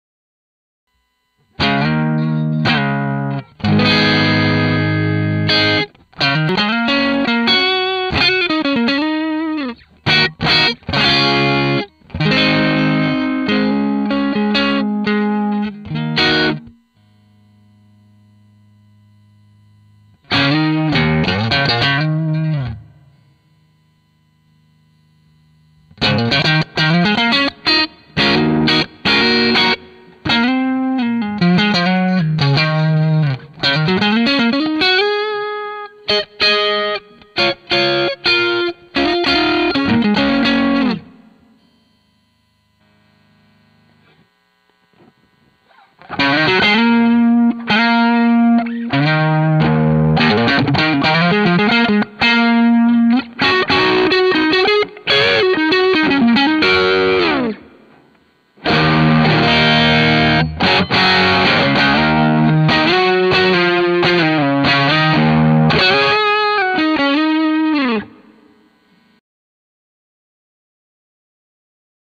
Marshall Plexi 1959 | Zoom G1 four Preset #06 [G1X four G3n G3Xn G5n]
Preset low-gain utilizando a simulação do amplificador Marshall Plexi 1959 na Zoom G1 four (MS 1959) plugada direto na interface. As configurações do preset são mostradas no final do vídeo.

[Low-Gain preset using the Marshal Plexi 1959 simulation on the Zoom G1 four (MS 1959) connected directly to the interface. The preset settings are shown at the end of the video.]

Patch Level: 80
Zoom G1 four Output: Lo: 0 | Mid: 0 | Hi: 0 | Vol: 100

Gear:
- Seizi Television (Eb Ab Db Gb Bb Eb)
- Malagoli Custom 84 / Custom 59
- Zoom G1 four (Marshall Plexi 1959)
- M-Audio Fast Track
- Reaper v6.10